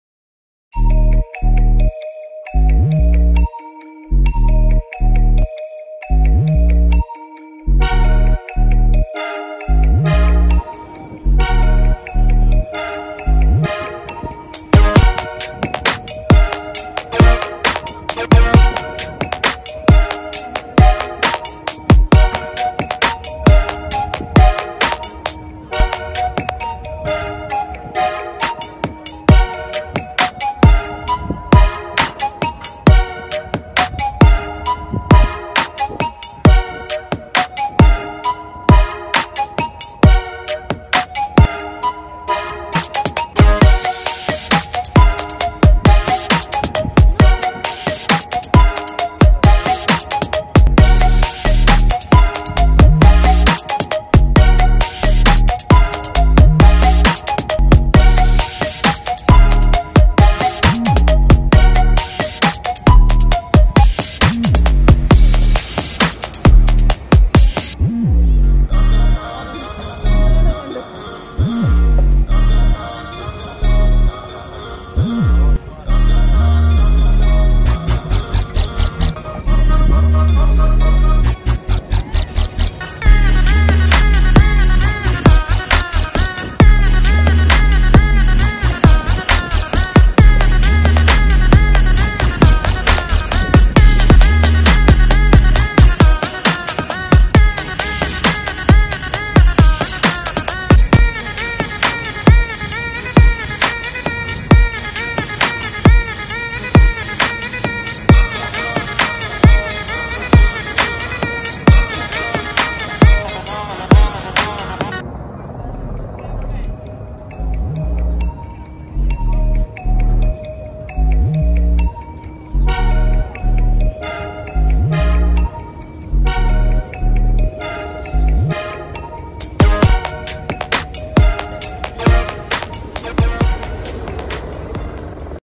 Amazing New Look Big Ben 2022
The National Landmark the Famous Big Ben New Refurbished it’s now open in spring 2022 for the Tourist it’s so lovely.located in the heart of Central London.